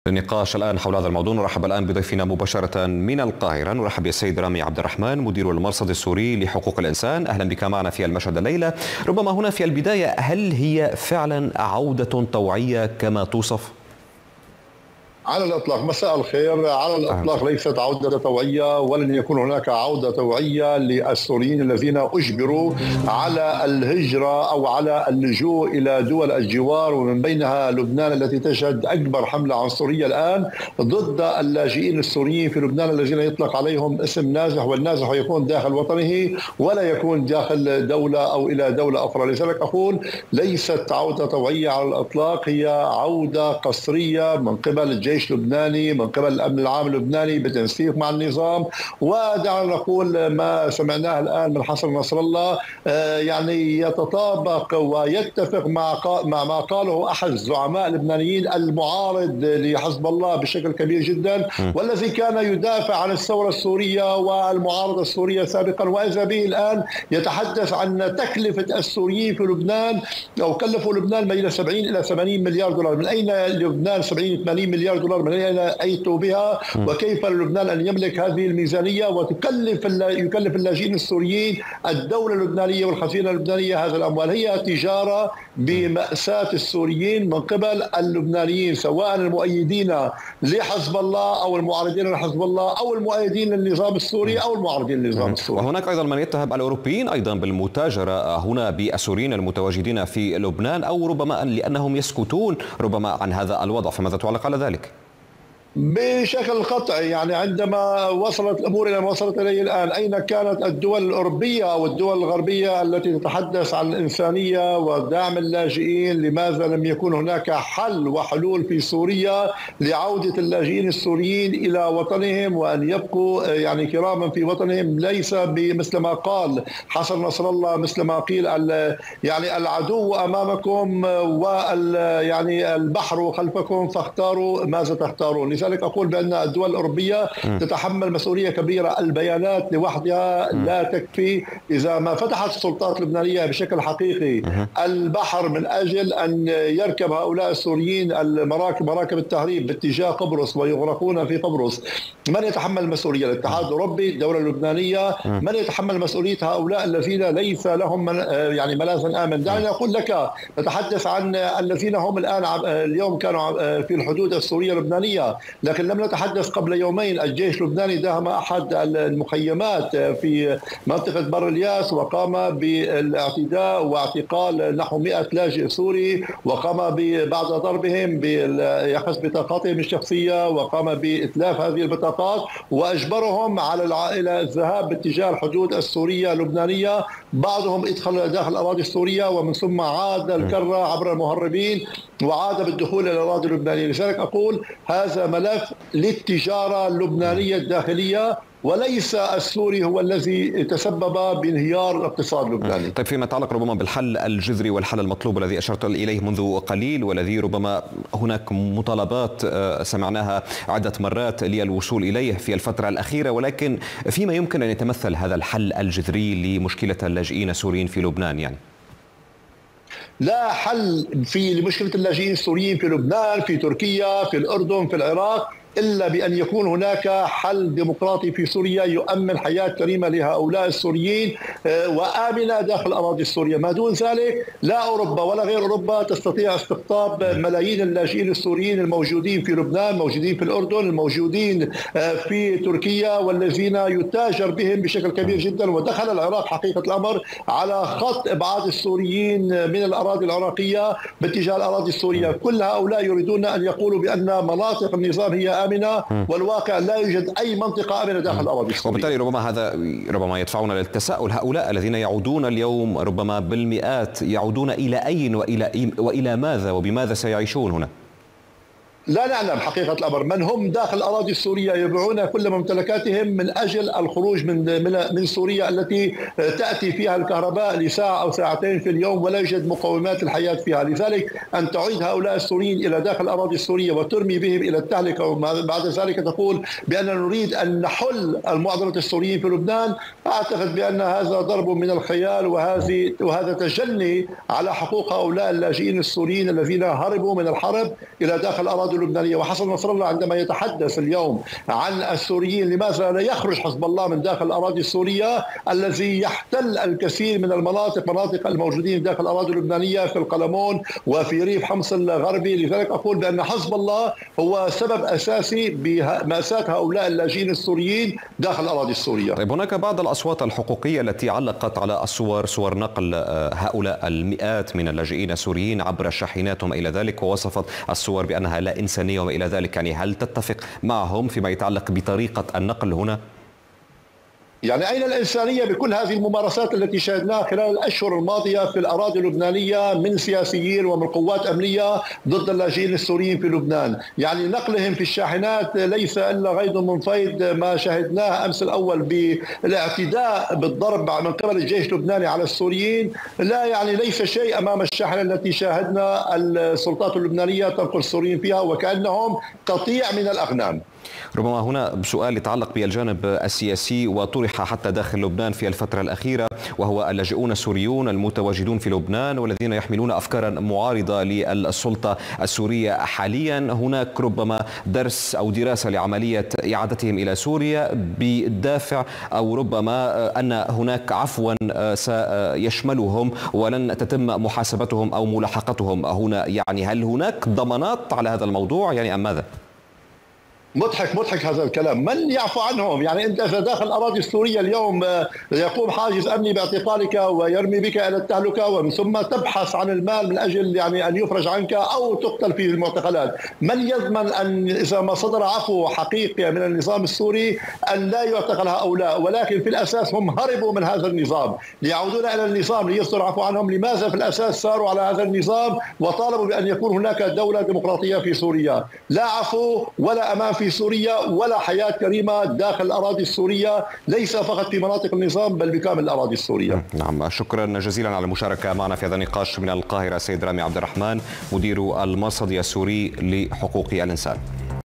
مدير المرصد_السوري: ما يجري بحق السوريين في لبنان هي تجارة بمأساتهم
مدير المرصد_السوري: ما يجري بحق السوريين في لبنان هي تجارة بمأساتهم بالتوازي مع حملة عنصرية ضدهم.. السلطات اللبنانية تدعي عودة السوريين بشكل طوعي وبالحقيقة هي تجبر السوريين على العودة وترتكب الانتهاكات بحقهم.
الحل الجذري لملف اللاجئين السوريين في دول الجوار إيجاد حل يؤمن حياة كريمة لهم في سورية.. والدول تريد القول أن سوريا بلد آمن ولكن الواقع مختلف.. ومن يضمن لهم أن يعودوا دون ملاحقة أمنية من النظام السوري.. والسوريون باعوا ممتلكاتهم ومنازلهم لمغادرة سورية.. وفي سورية لا وجود لمقومات الحياة.